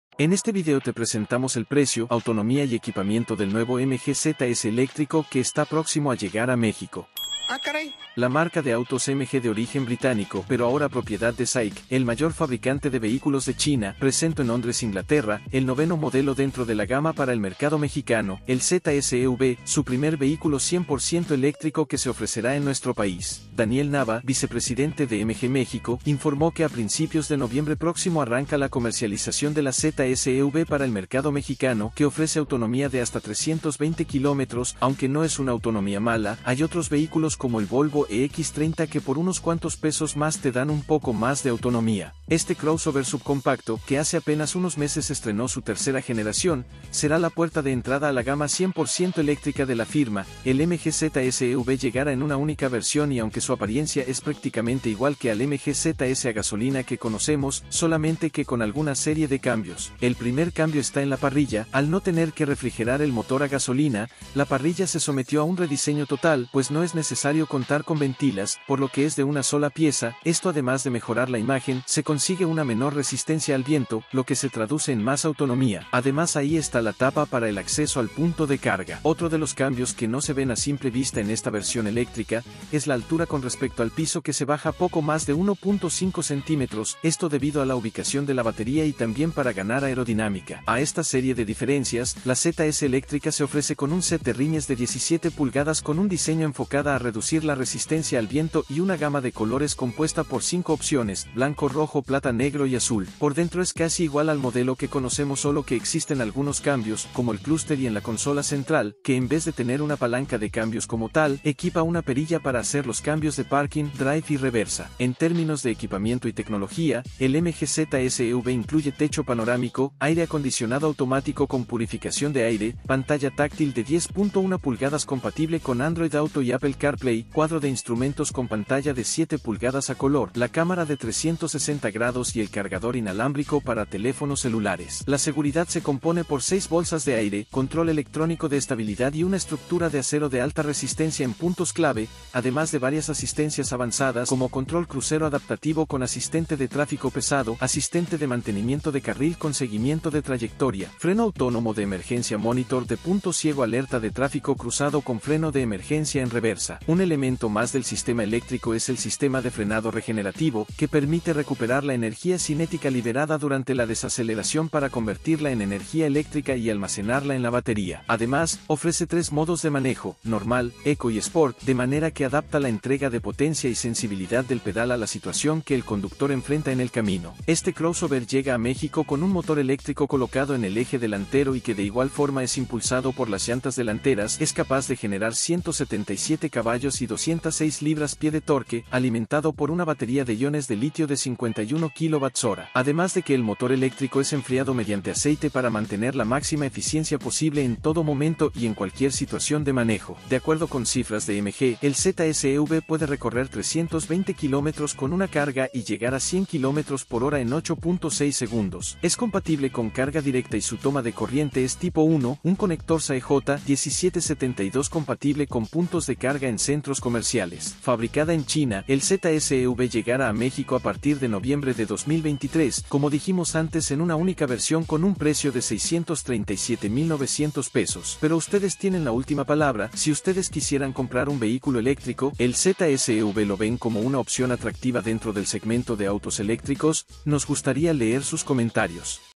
MG ZS EV 2024 EL PRIMER MG 100% ELÉCTRICO EN MÉXICO
La marca de autos MG presento en Londres Inglaterra el noveno modelo dentro de la gama para el mercado mexicano: el ZS EV, su primer vehículo 100 % eléctrico que se ofrecerá en nuestro país.

Su precio de entrada en su única versión arranca desde $637,900 MXN.

Saludos.